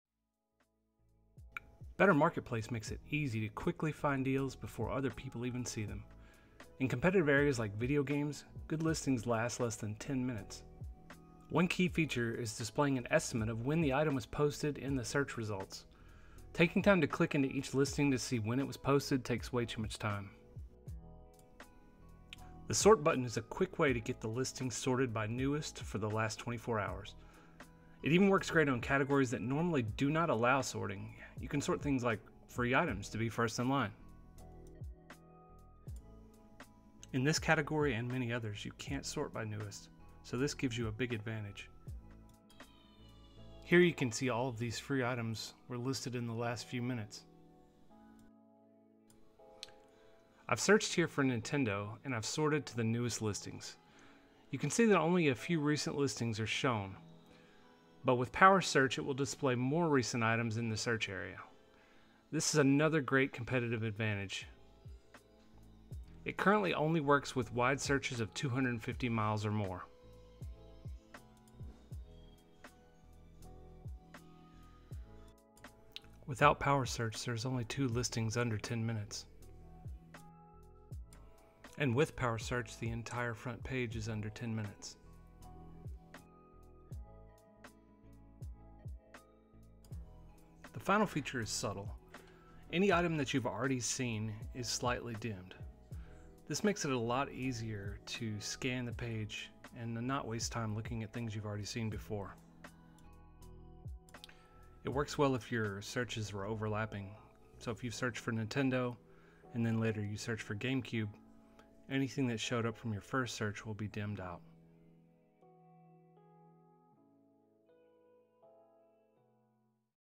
Better Marketplace Pro - Improve Facebook Marketplace sorting and find the newest listings
In the fast-paced world of online deals, timing is everything. Better Marketplace Pro is designed to optimize your deal hunting on Facebook Marketplace, ensuring you never miss out on a great deal. Our extension helps you identify new listings quickly, streamlining your search and buying process.

It includes the ability to power search listings and sort categories that you wouldn't normally be able to sort.